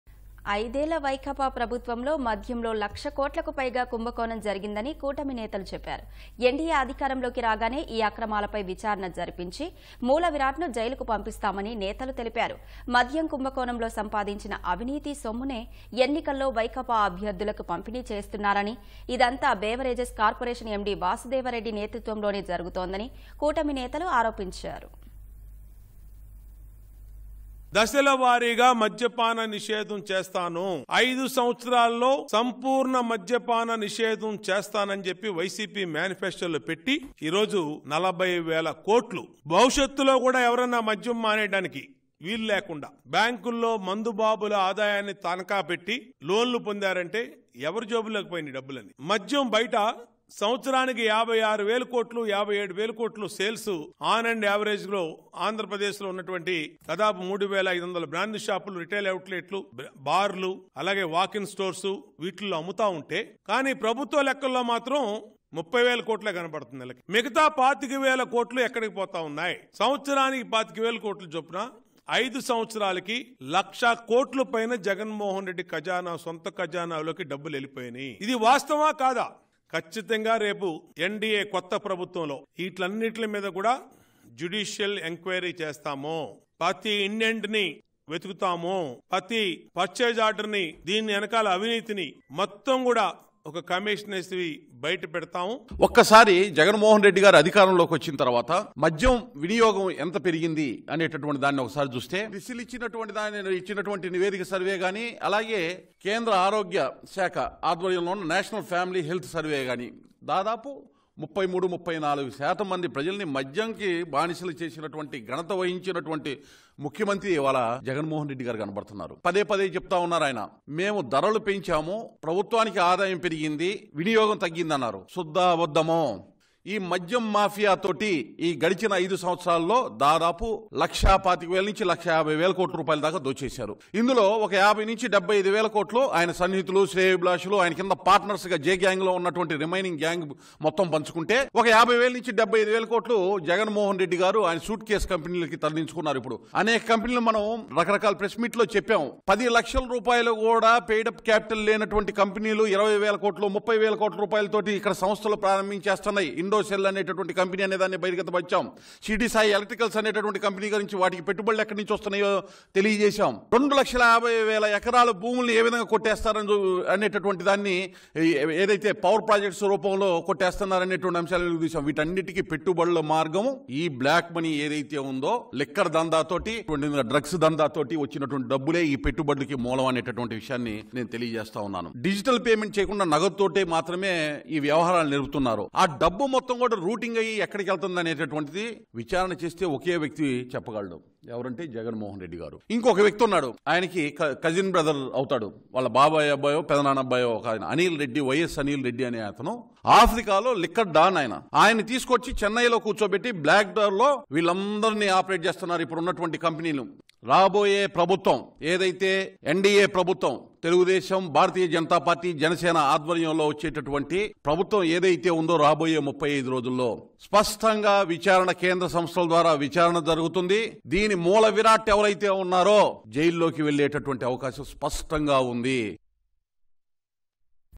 ఐదేళ్ల వైకాపా ప్రభుత్వంలో భారీ మద్యపాన కుంభకోణం జరిగింది | Big Fraud In Alcohol Sector In YCP Govt
ఐదేళ్ల వైకాపా ప్రభుత్వంలో మద్యంలో లక్ష కోట్లకు పైగా కుంభకోణం జరిగిందని... కూటమి నేతలు చెప్పారు. NDA అధికారంలోకి రాగానే ఈ అక్రమాలపై విచారణ జరిపించి... మూలవిరాట్ ని జైలుకి పంపిస్తామని నేతలు తెలిపారు. మద్యం కుంభకోణంలో సంపాదించిన అవినీతి సొమ్మునే ఎన్నికల్లో వైకాపా అభ్యర్థులకి పంపిణీ చేస్తున్నారని... ఇదంతా బేవరేజెస్  కార్పొరేషన్ ఎండీ వాసుదేవరెడ్డి నేతృత్వంలోనే జరుగుతోందని కూటమి నేతలు ఆరోపించారు...